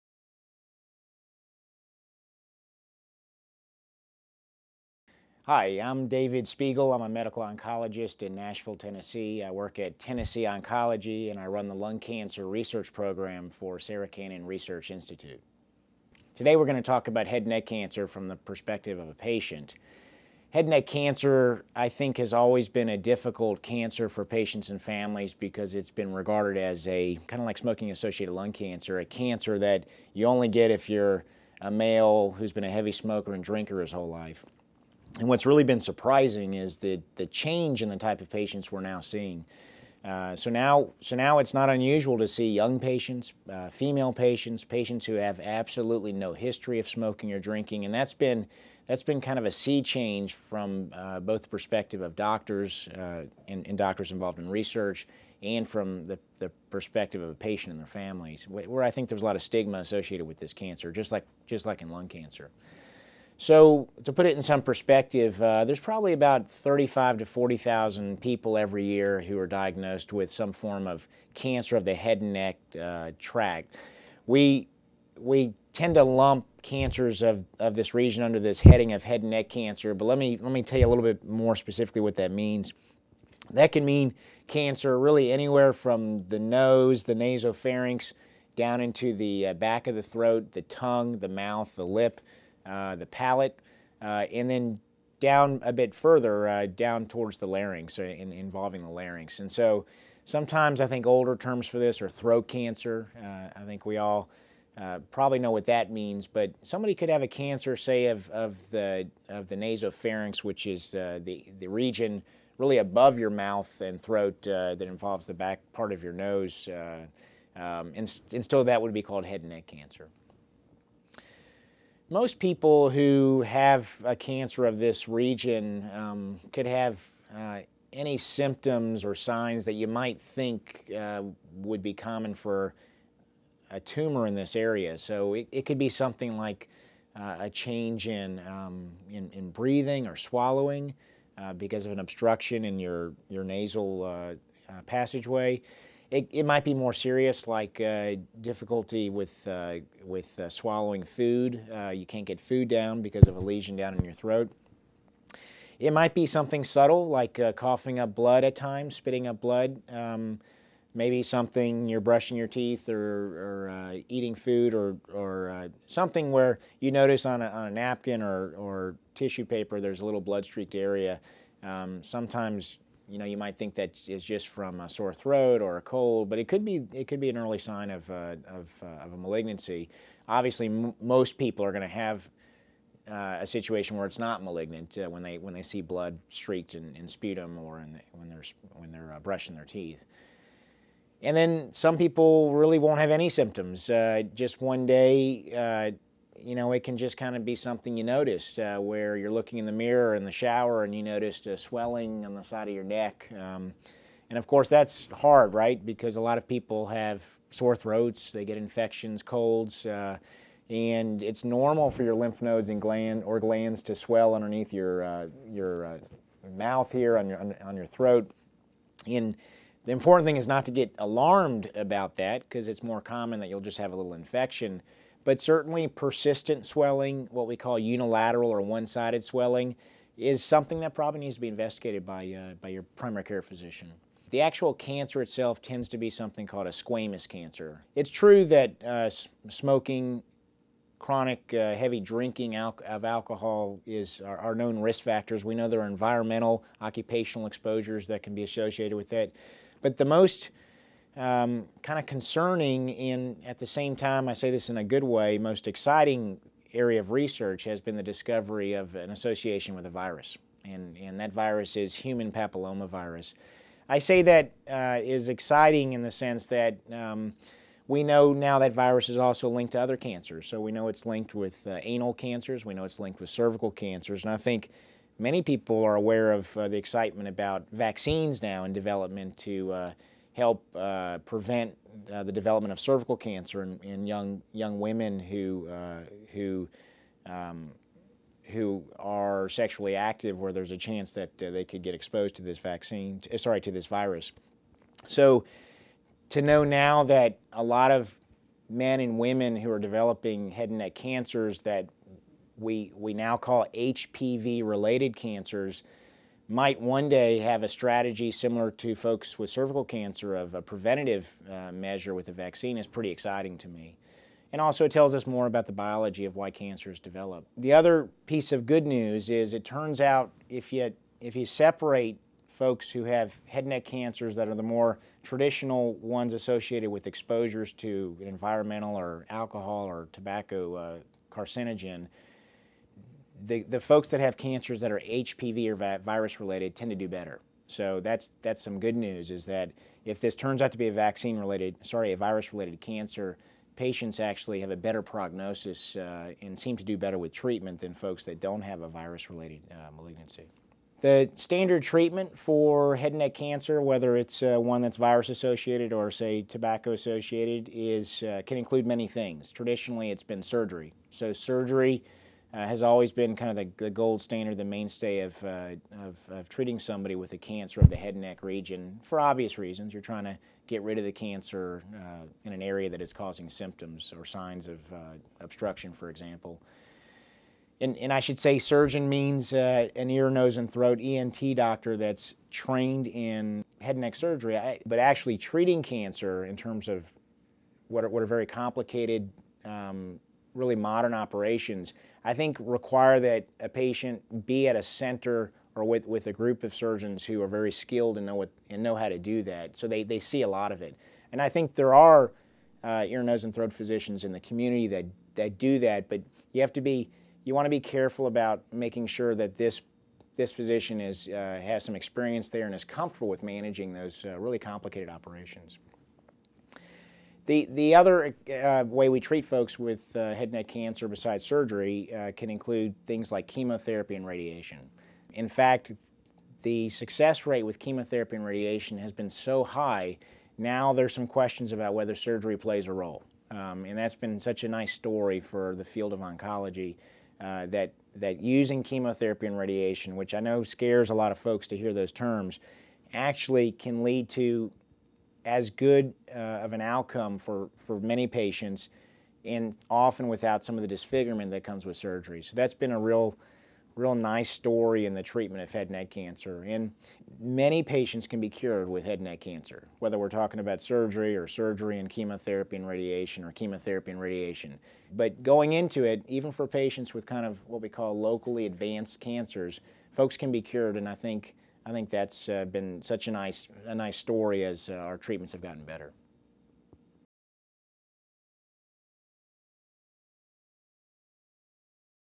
Head and Neck Cancer: Causes, Symptoms, and Treatment
David Spigel, M.D., defines head and neck cancer, its symptoms and breakthroughs in its treatment.